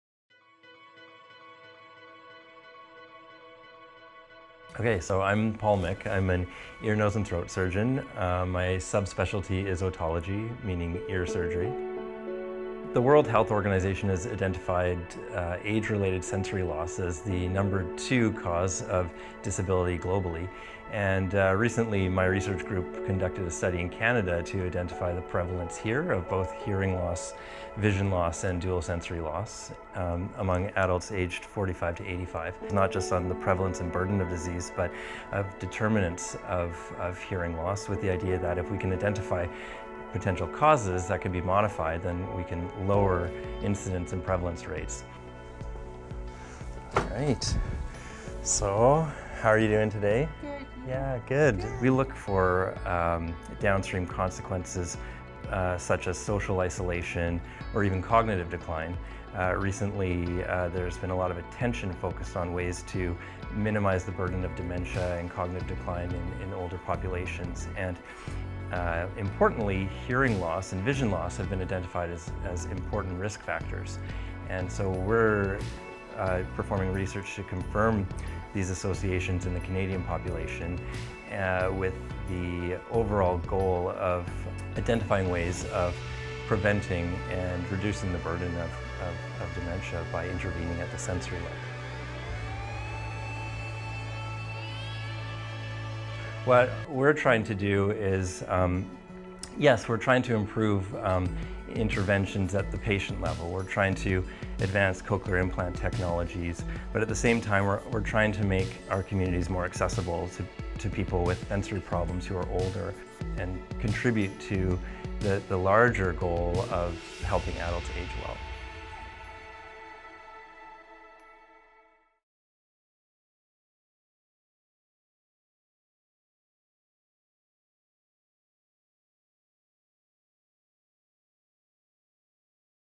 Preventing Hearing and Vision Loss to Ease Dementia | Dr. Paul Mick (MD)
Hearing and vision loss are associated with faster cognitive decline in healthy older adults, and also an increased risk of developing dementia. Ear, nose and throat surgeon Dr. Paul Mick (MD) explains how his research group is looking to identify the reasons underlying the associations, and how to prevent and reduce sensory-loss related challenges for older adults, including people living with dementia. Mick is a surgeon in the Saskatchewan Health Authority and an associate professor within the Department of Surgery in the College of Medicine.